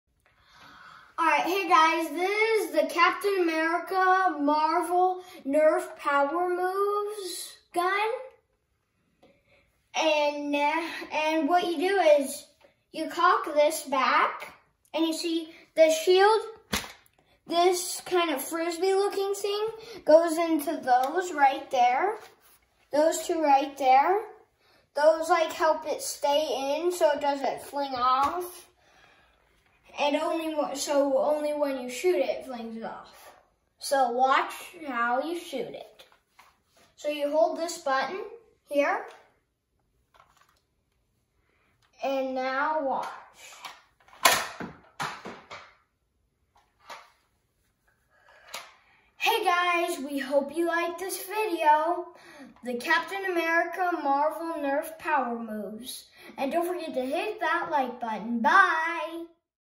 Captain America Nerf Power Move Toy Review
Kalifornia Kid demonstrates the Captain America Nerf power move weapon.